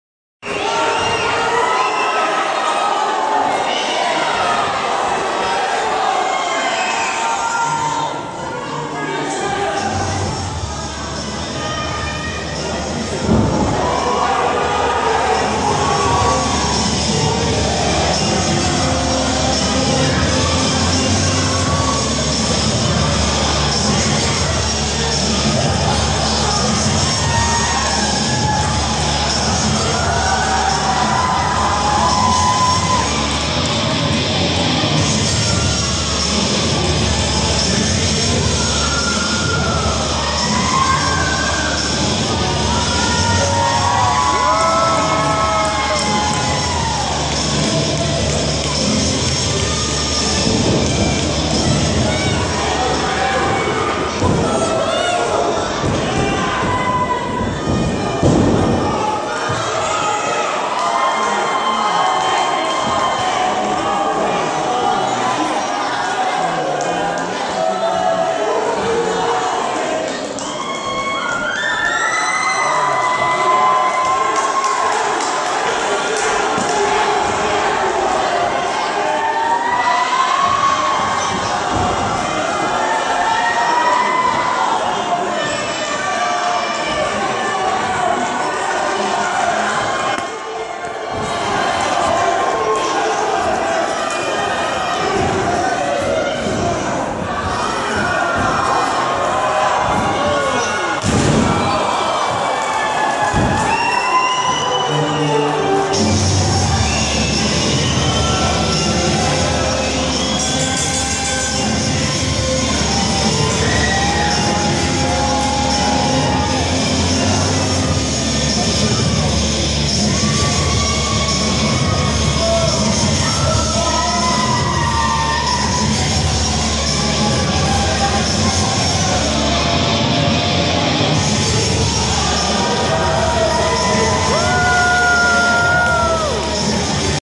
Monster Abyss Comes To WOH Wrestling
October 30th Monster Abyss made a suprise appearance at WOH Wrestling.  This did not end well for Eric Alden. Blood, tacs, and tables.